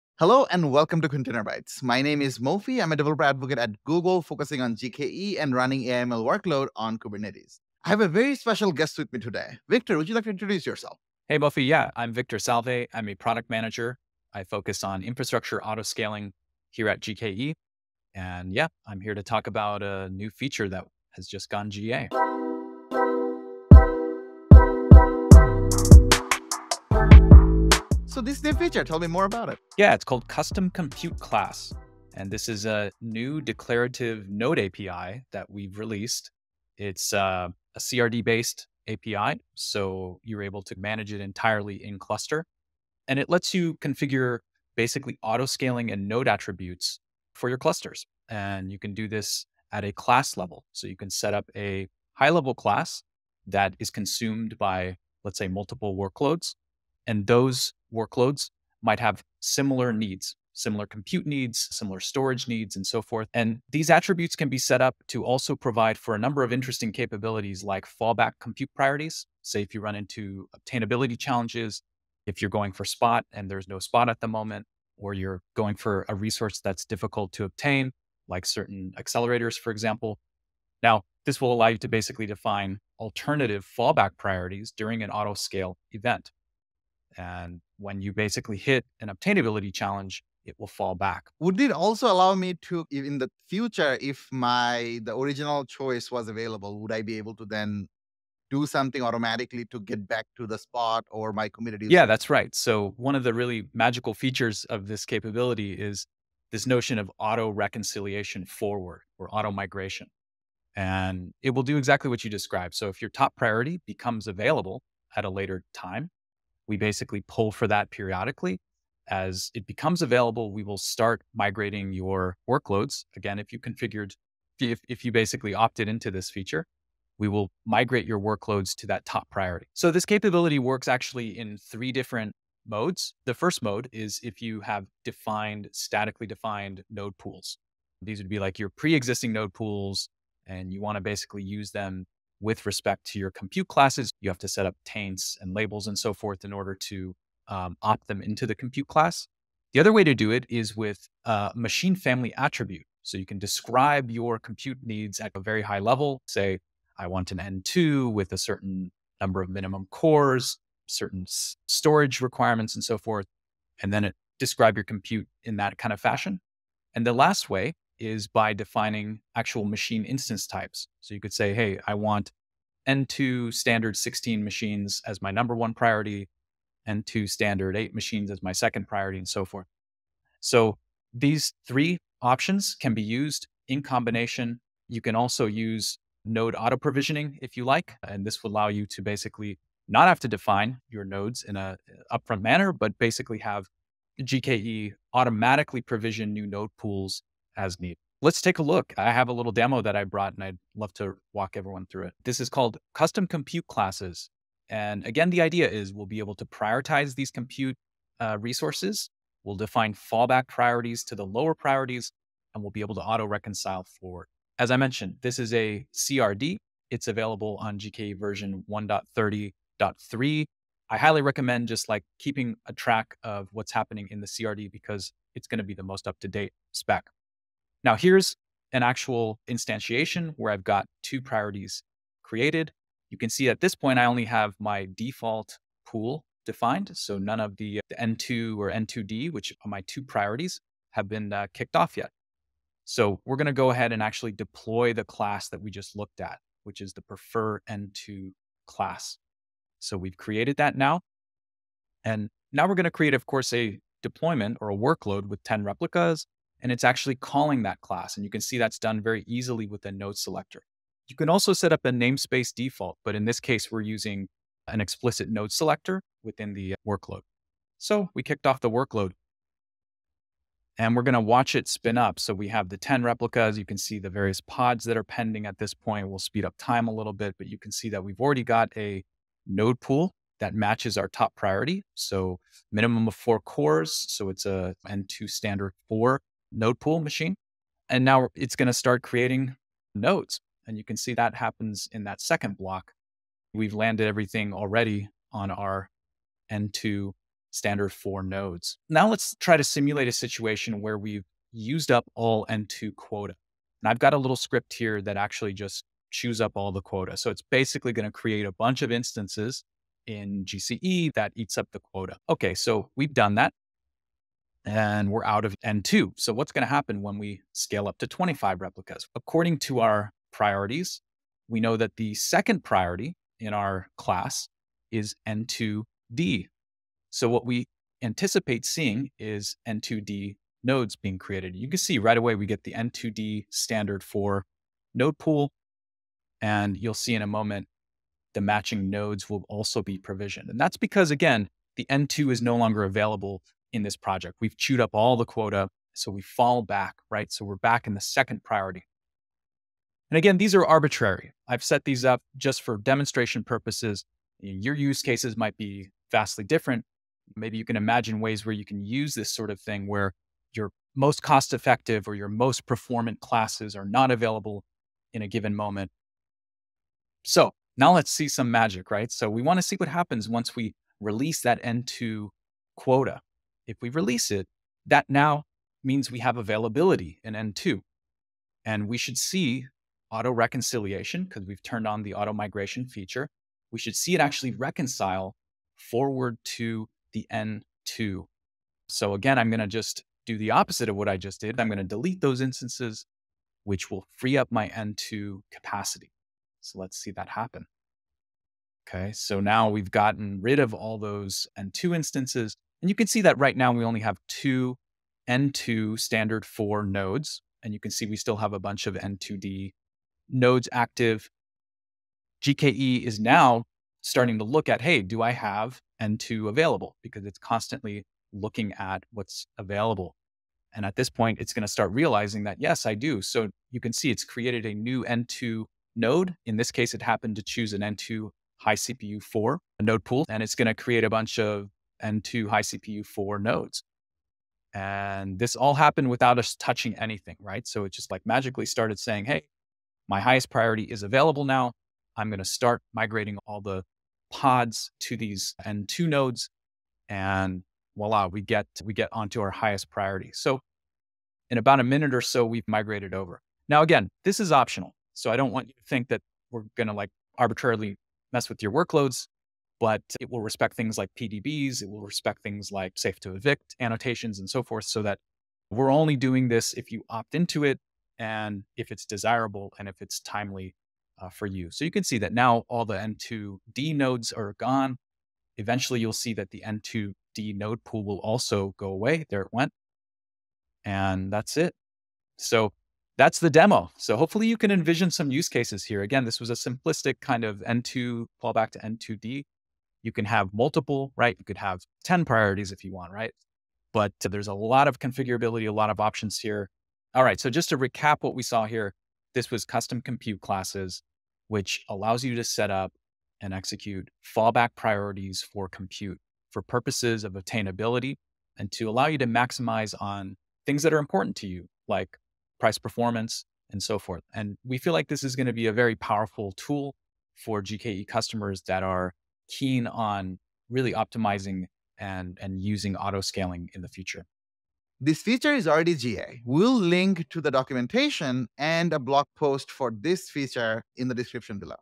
Improve Infrastructure Autoscaling with Custom Compute Classes in GKE
In this video we'll explore the new custom compute class feature in GKE, which improves infrastructure autoscaling with fall-back compute priorities and advanced node configuration in a new declarative, CRD-based API.